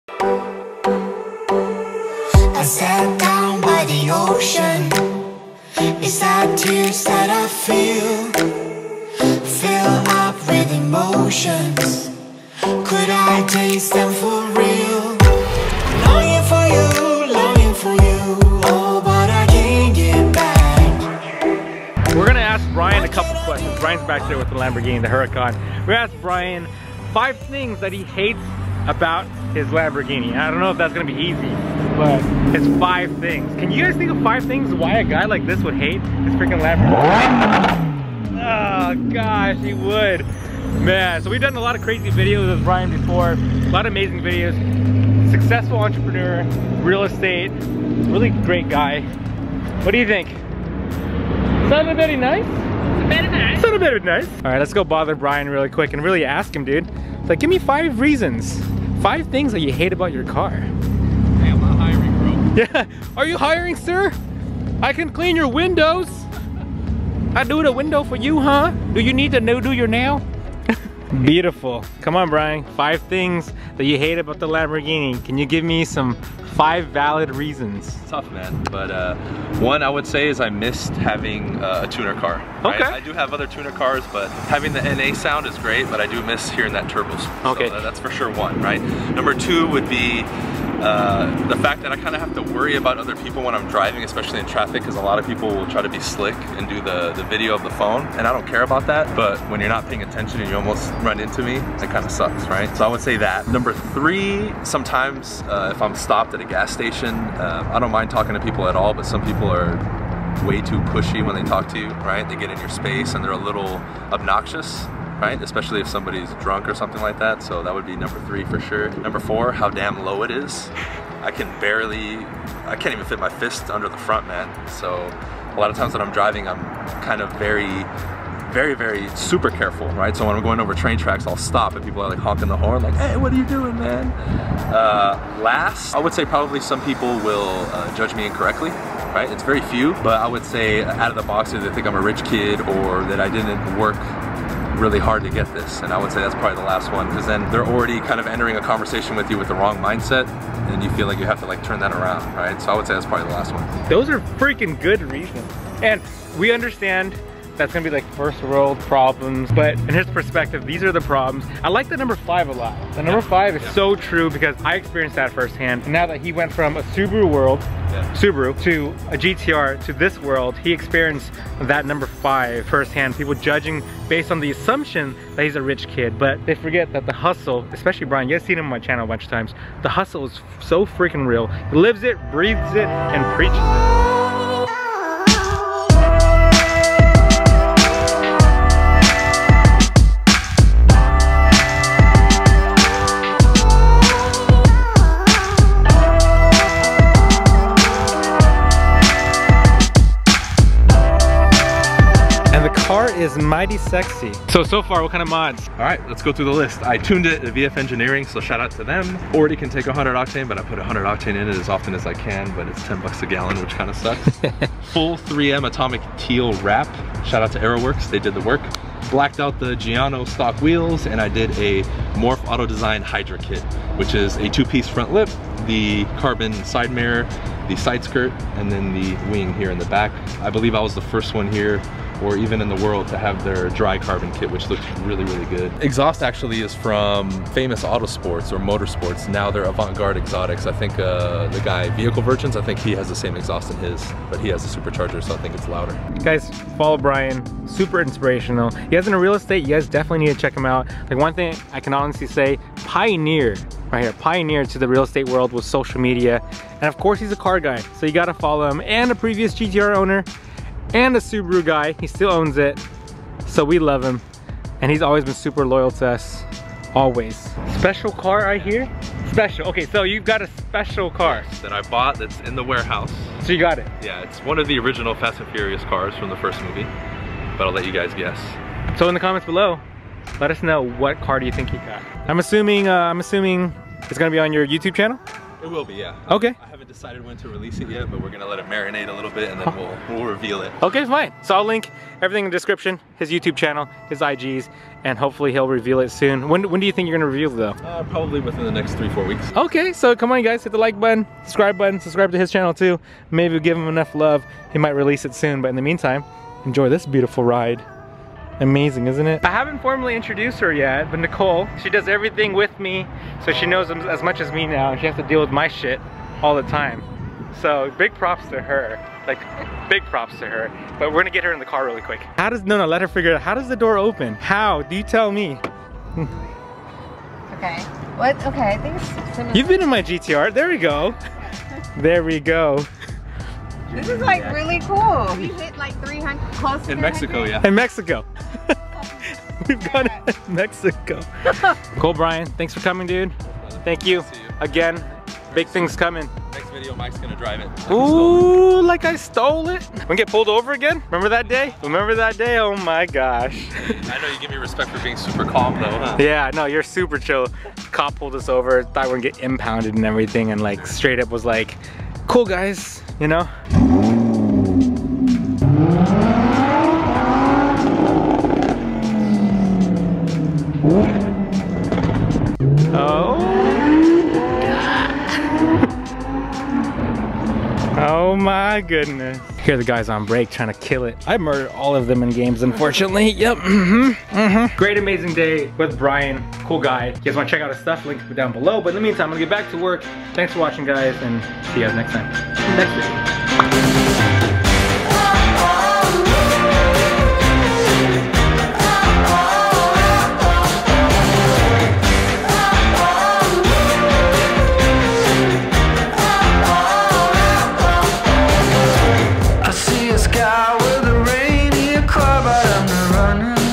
5 THINGS I HATE ABOUT THE LAMBORGHINI HURACAN
This Huracan is amazing so it was really hard for Bryan to find 5 reasons on why he would hate this car!

The Camera I use
►►Add me on SnapChat: @SmurfinWRX

! ► FAN MAIL ► !
Michael Primal Driven
1100 S Grove Ave
Suite E-2

►Gaming Channel!

Primal Driving Clothing